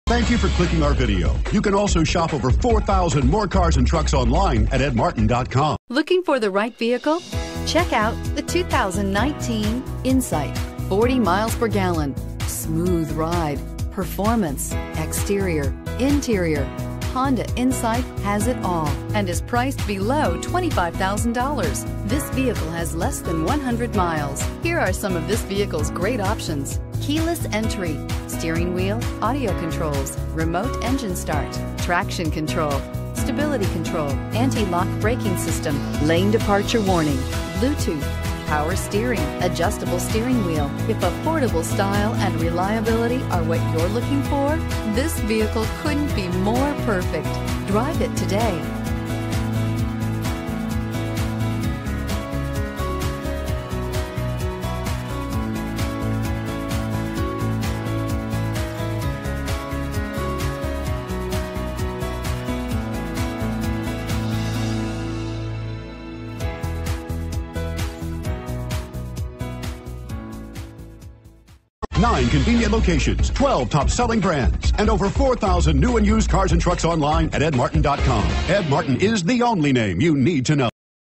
2019 Honda Insight 2IN1359
2019 HONDA INSIGHT Indianapolis, IN
Stock# 2IN1359

HYBRID, BACK-UP CAMERA, LANE DEPARTURE WARNING, BLUETOOTH, MP3 Player, KEYLESS ENTRY, 49 MPG Highway, ALLOY WHEELS.